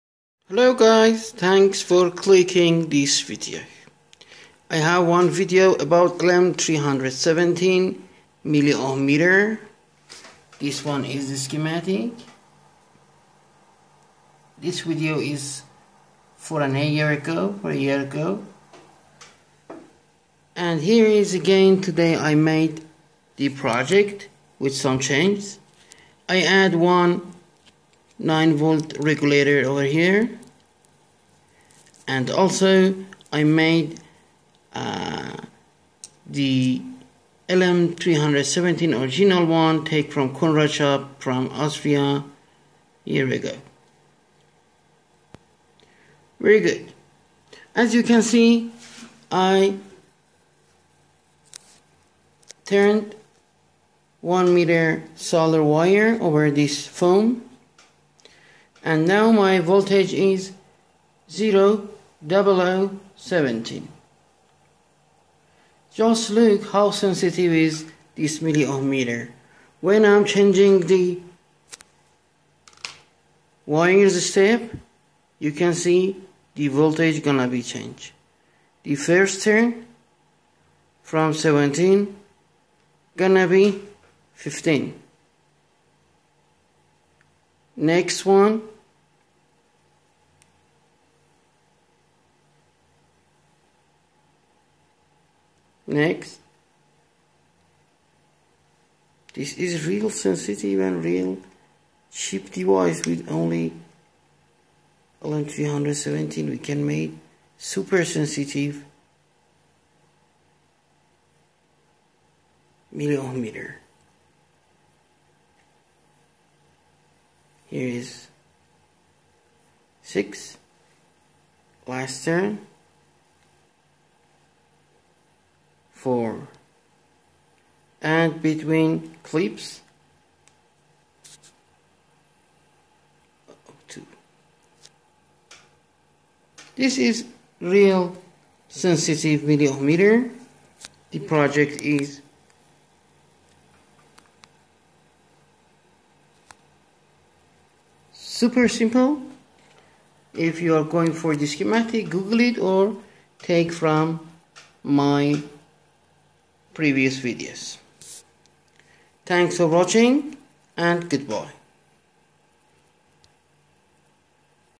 LM317 Milliohm meter it too sensitive than able to measure wire resistance.
4 video I have about milliohm meter with Lm317
today I decide to make some changes and here is the result:
This project could be very good to find Circuit board shorts or long and small wires shorts.